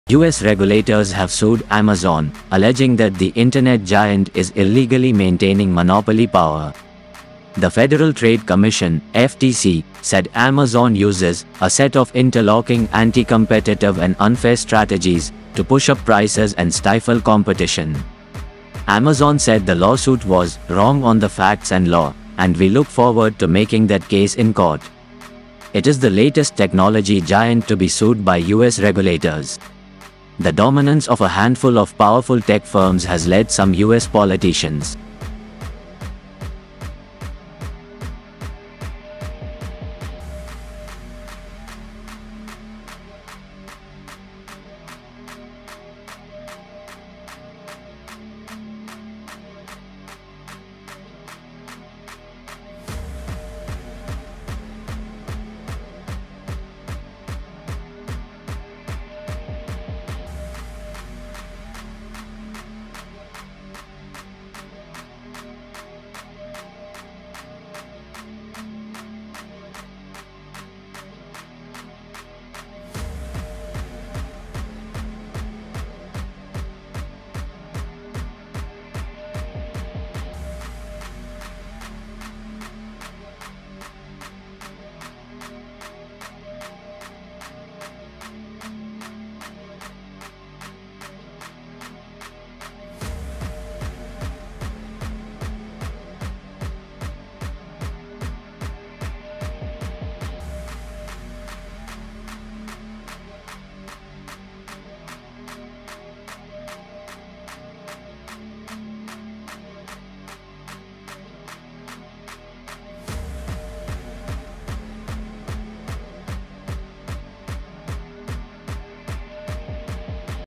Amazon: US accuses online giant of monopoly FTC files a massive antitrust
US regulators have sued Amazon  that the internet giant is maintaining monopoly power.

The Federal Trade Commission (FTC) said Amazon uses "a set of interlocking anticompetitive and unfair strategies" to push up prices and stifle competition.

Amazon said the lawsuit was "wrong on the facts and law, and we look forward to making that case in court".

It is the latest technology giant to be sued by US regulators.

The dominance of a handful of powerful tech firms has led some US politicians